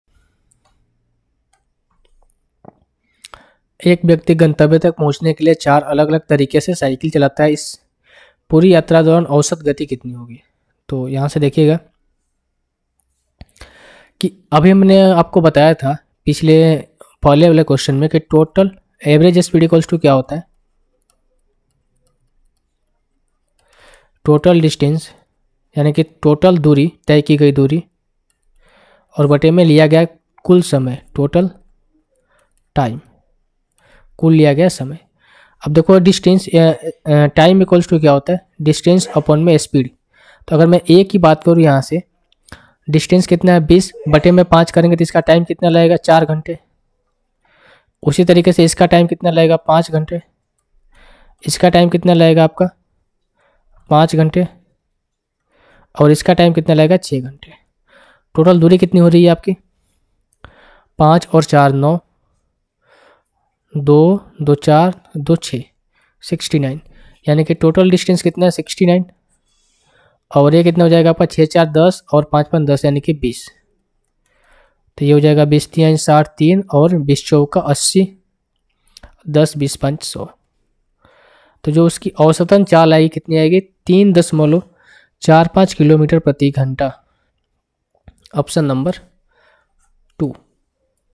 A person rides bicycle on four different ways to reach the destination. Determine the average speed
A person rides bicycle on four different ways to reach the destination. Determine the average speed during this whole journey एक व्यक्ति गंतव्य तक पहुंचने के लिए चार अलग-अलग तरीकों से साइकिल चलाता है। इस पूरी यात्रा के दौरान औसत गति ज्ञात कीजिए